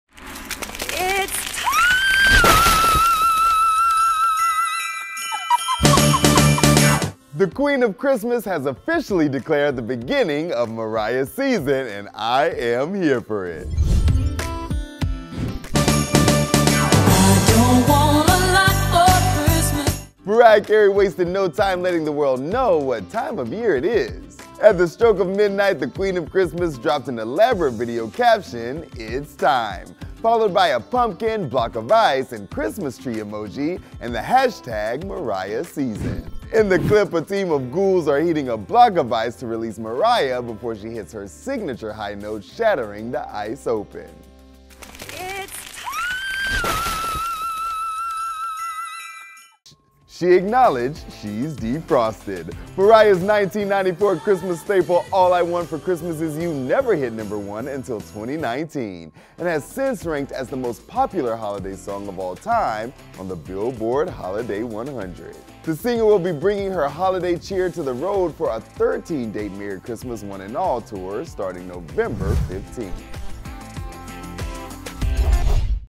Mariah Carey Kicks Off The Christmas Season: ‘It’s Time’ | Billboard News
The queen of Xmas, Mariah Carey, defrosted the holiday at the stroke of midnight on November 1st.